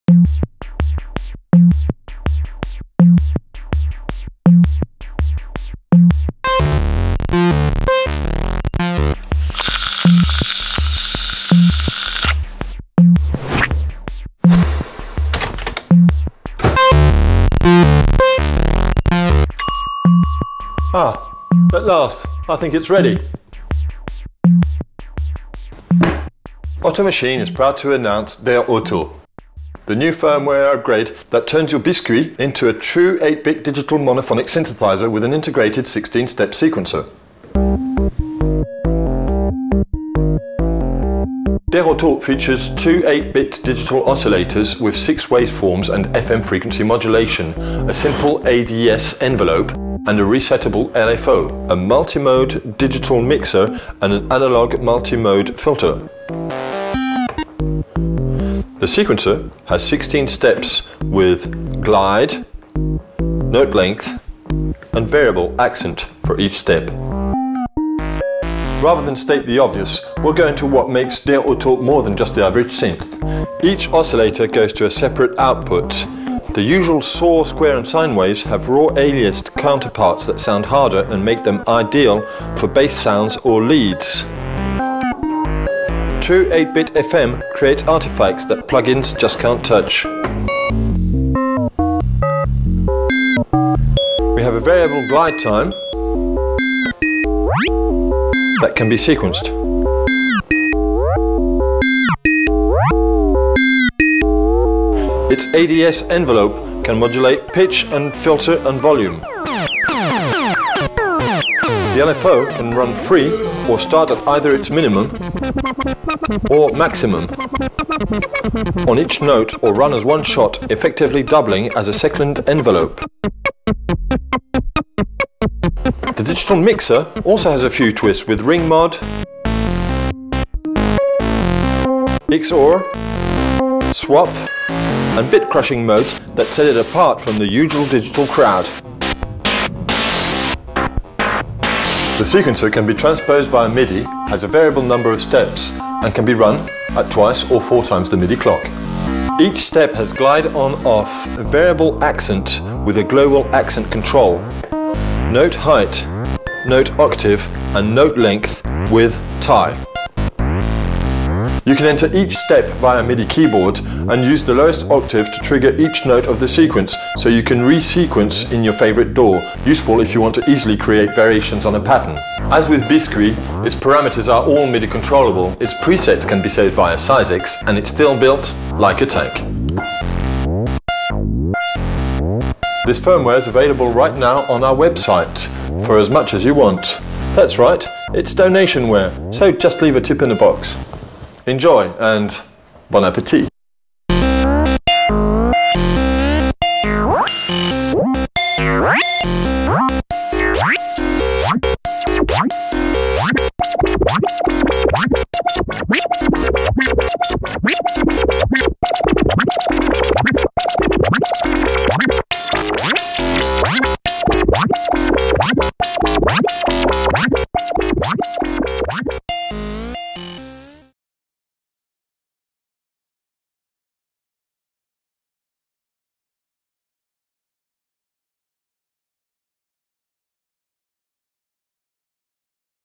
DER OTO
Der OTO is the new firmware upgrade that turns your Biscuit into a 2 oscillators monosynth with an integrated 16 steps sequencer.